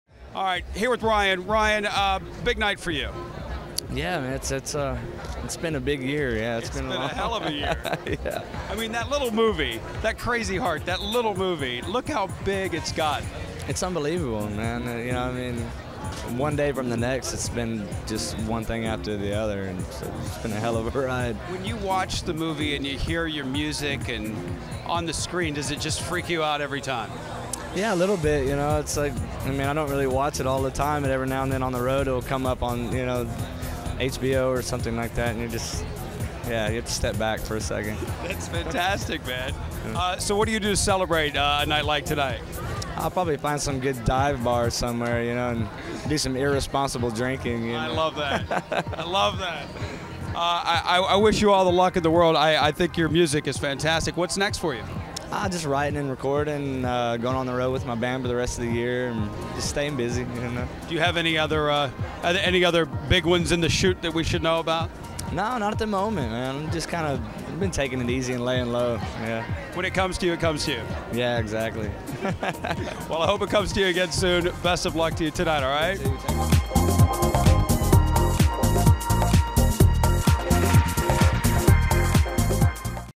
53rd Grammy Awards - Ryan Bingham Interview
Ryan Bingham Red Carpet Interview with Chris Booker